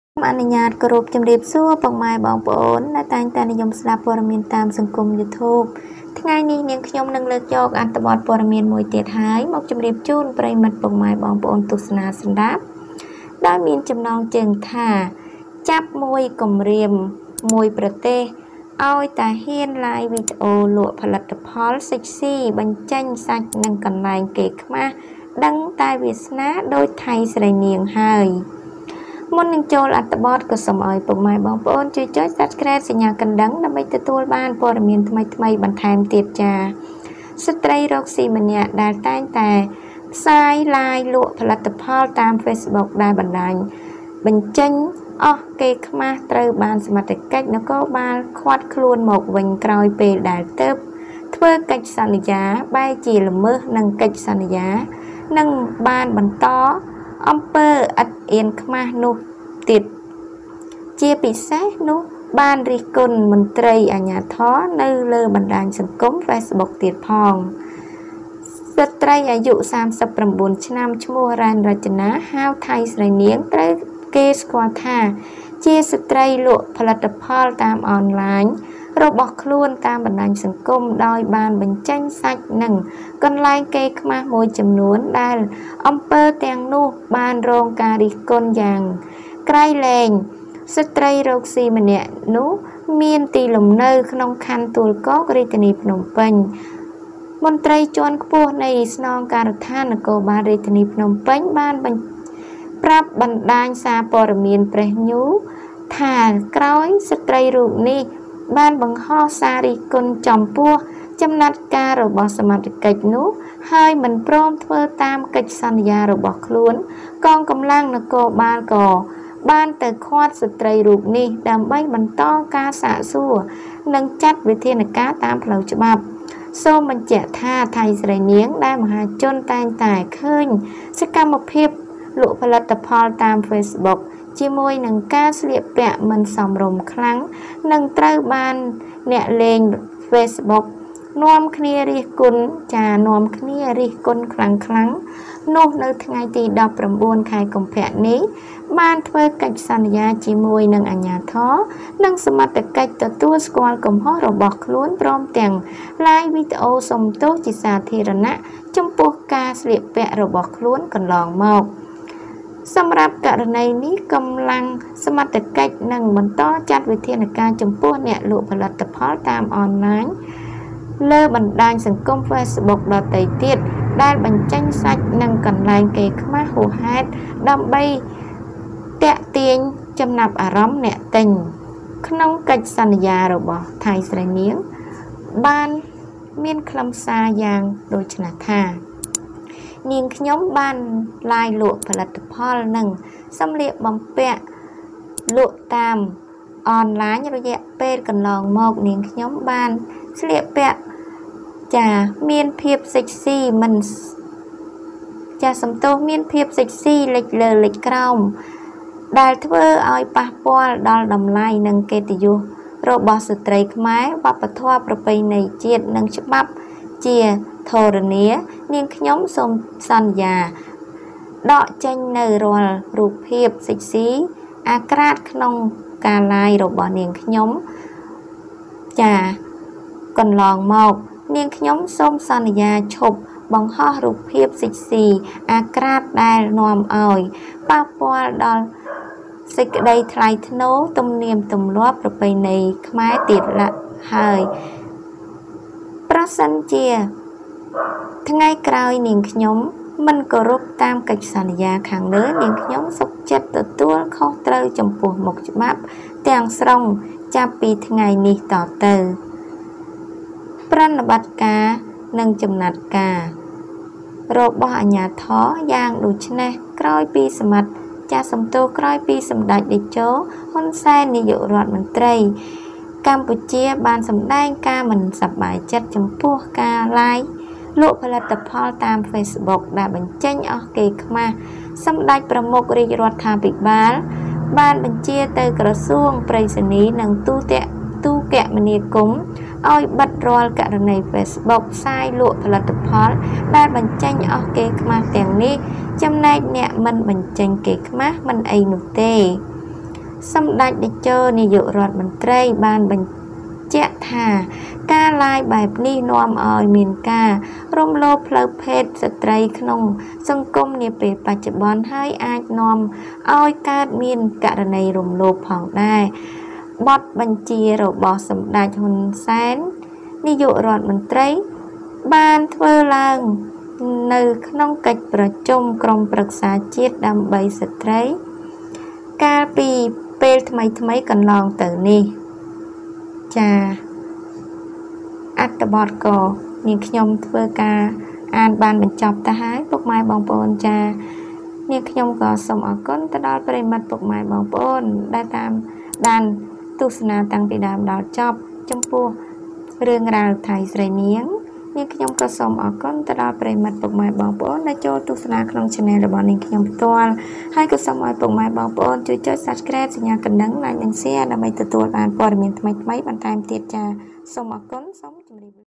ចាប់ ១ គំរា មមួយប្រទេស ! ឲ្យតែហា៊ន live វិដេអូ លក់ផលិតផល សុិ ច សុី បញ្ចេញសាច់ និងកន្លែងកេរ្តិ៍ខ្មាស
Catch a Country Project!  As long as Hain live video sells sex products and places of shame as Thai, Srey Neang

ចាប់ ១ គំរា មមួយប្រទេស ! ឲ្យតែហា៊ន live វិដេអូ លក់ផលិតផល សុិ ច សុី បញ្ចេញសាច់ និងកន្លែងកេរ្តិ៍ខ្មាស ដឹងតែវាសនា ដូច ថៃ ស្រីនាង ហើយ

channel/ព័័ត៌មានចំរុះ